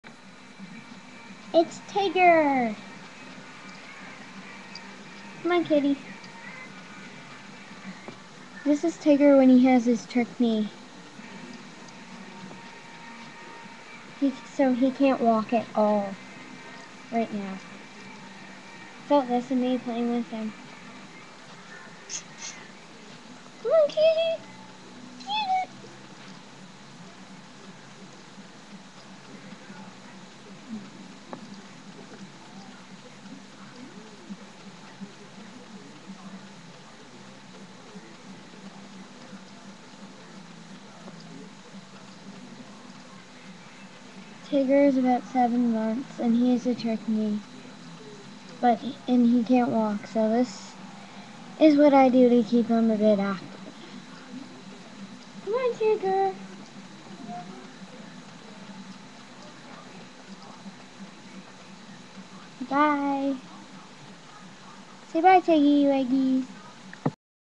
tigger milliken
this is my cat at 7 months when he has a trick knee